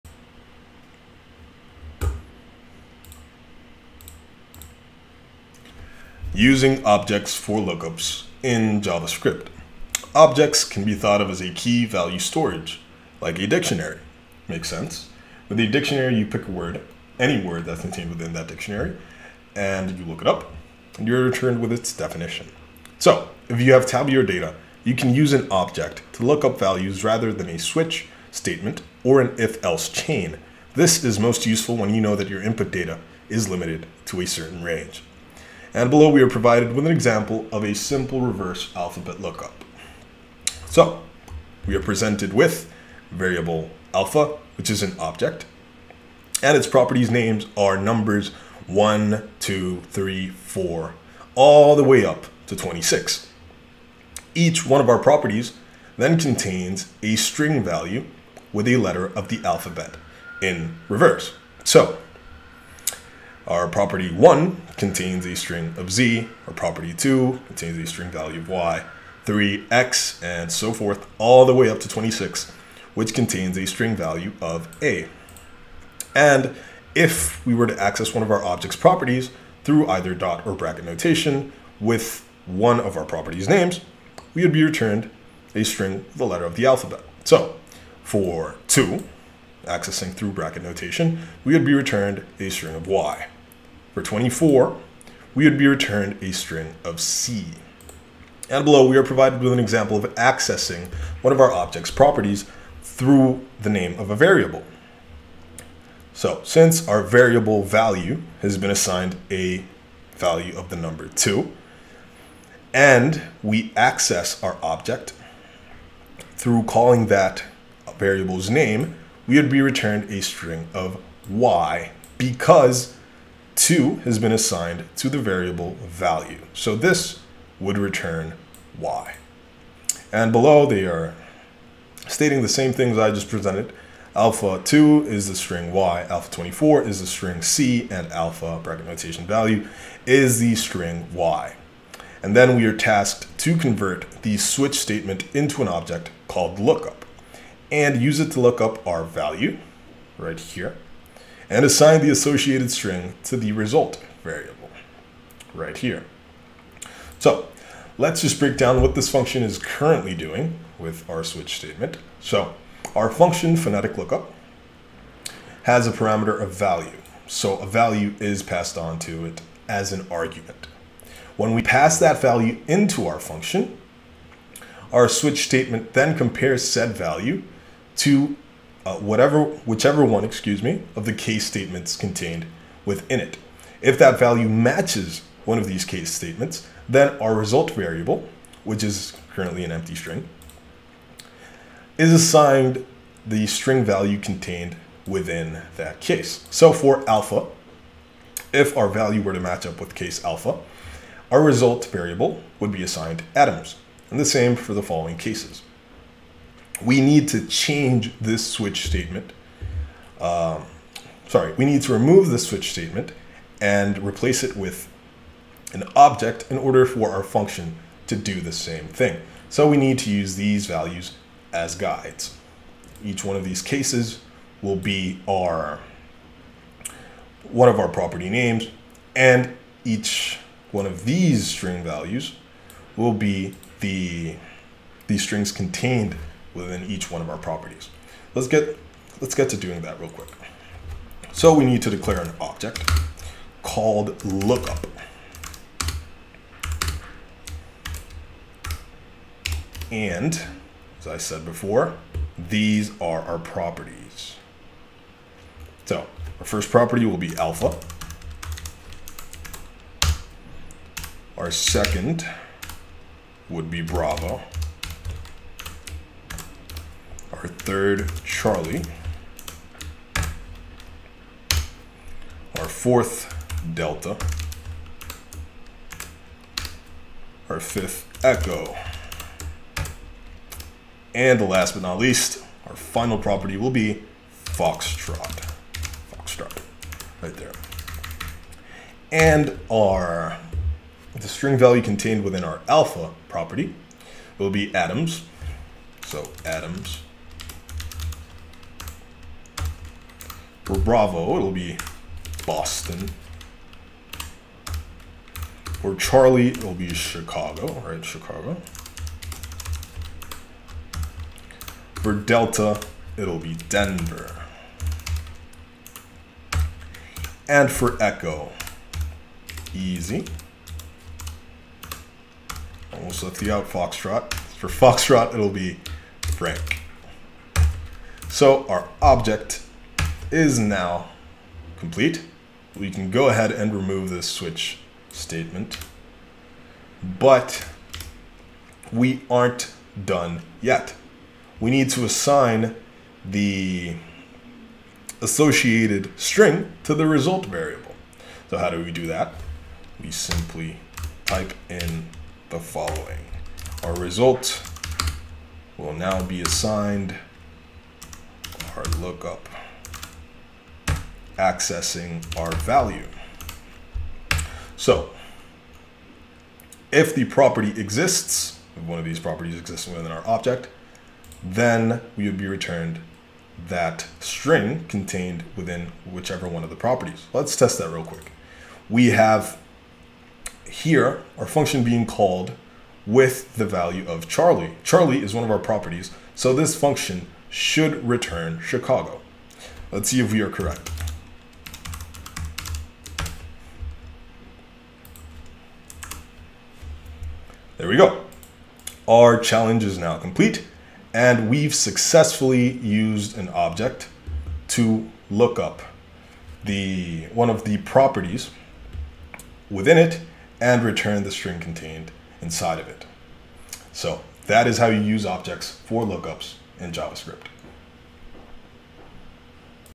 Using Objects for Lookups in JavaScript
By watching this video, you will learn how to use JavaScript objects to look up a value contained within that object's properties.